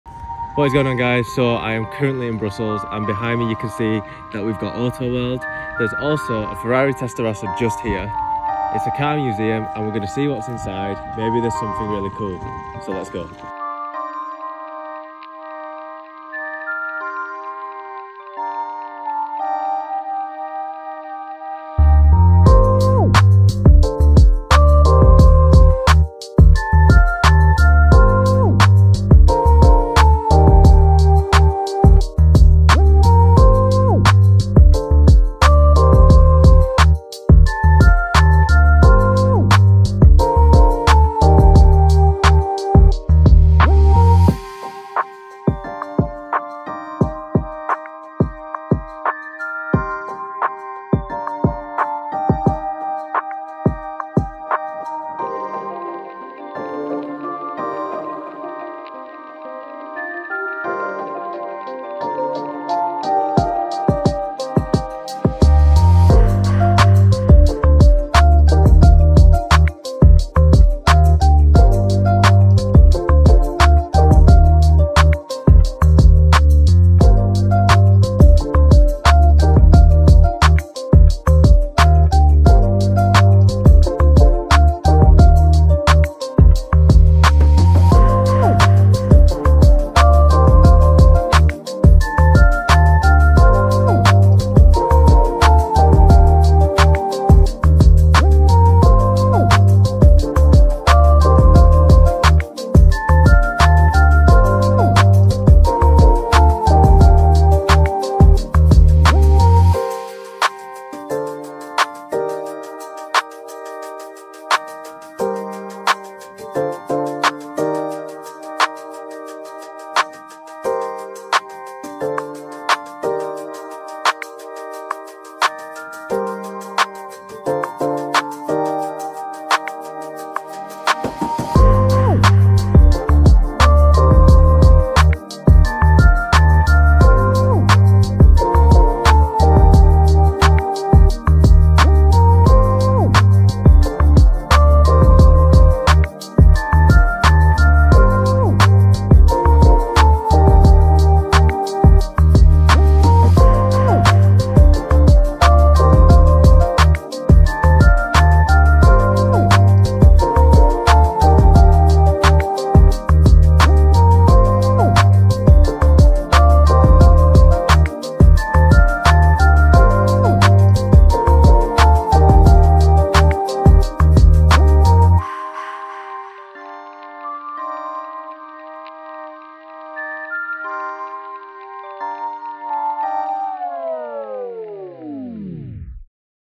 Autoworld Museum Tour in Brussels (July 2023)
Join me as I take you on a tour around Autoworld car museum in Brussels, Belgium!

Follow me for more Automotive car photography images, tips and tricks and content!